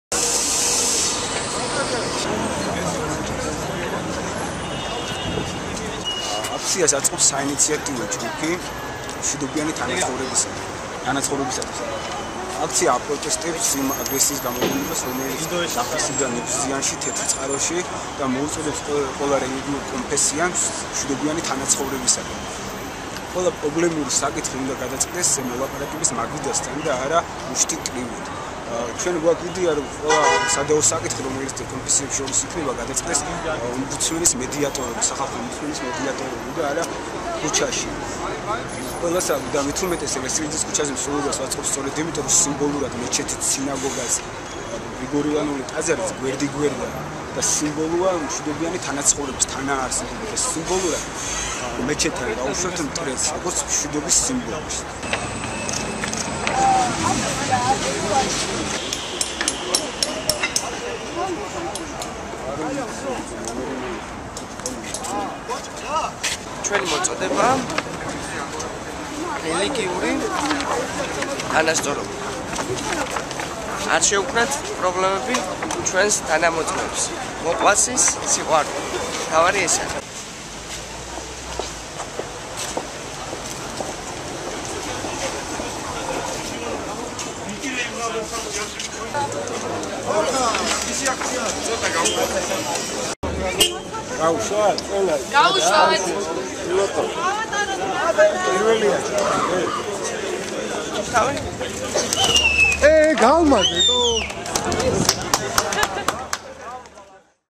სოლიდარობის აქცია
სტუდენტების საინიციატივო ჯგუფის ორგანიზებით სოფელ ნიგვზიანში და წინწყაროში მომხდარ ფაქტებთან დაკავშირებით სოლიდარობის აქცია და მსვლელობა გაიმართა